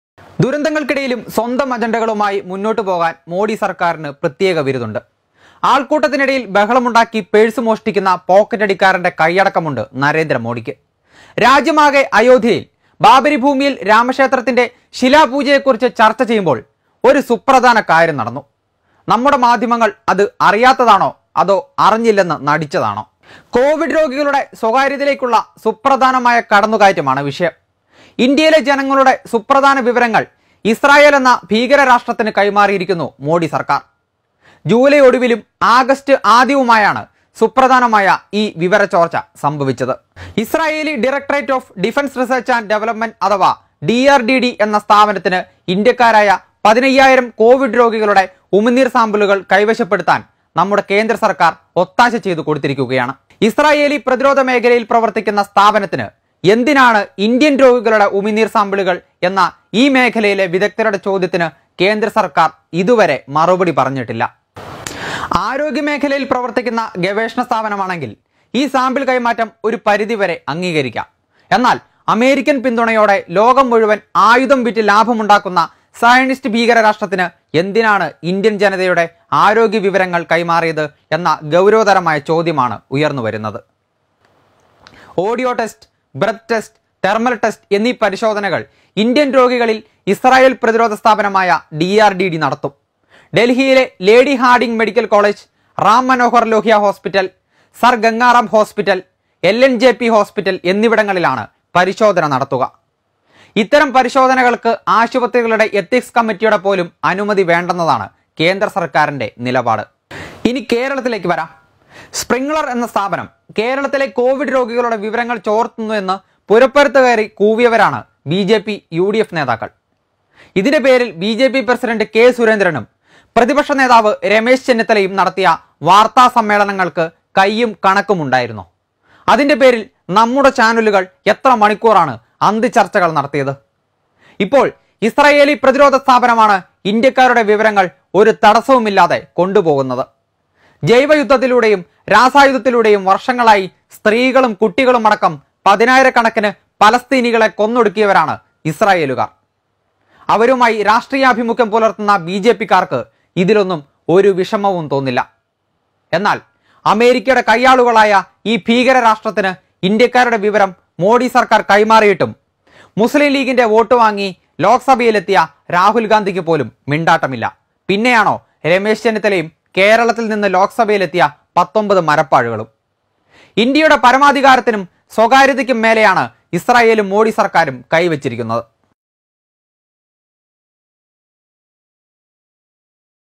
കോവിഡ് രോഗികളുടെ സ്വകാര്യതയിലേക്കുള്ള സുപ്രധാനമായ കടന്നുകയറ്റമാണ് വിഷയം
രാജ്യമാകെ അയോധ്യയിൽ ബാബരി ഭൂമിയിൽ  രാമക്ഷേത്രത്തിന്റെ ശിലാപൂജയെക്കുറിച്ചു ചർച്ച ചെയ്യുമ്പോൾ ഒരു സുപ്രധാന കാര്യം നടന്നു. നമ്മുടെ മാധ്യമങ്ങൾ അത് അറിയാത്തതാണോ അതോ അറിഞ്ഞില്ലെന്ന് നടിച്ചതാണോ.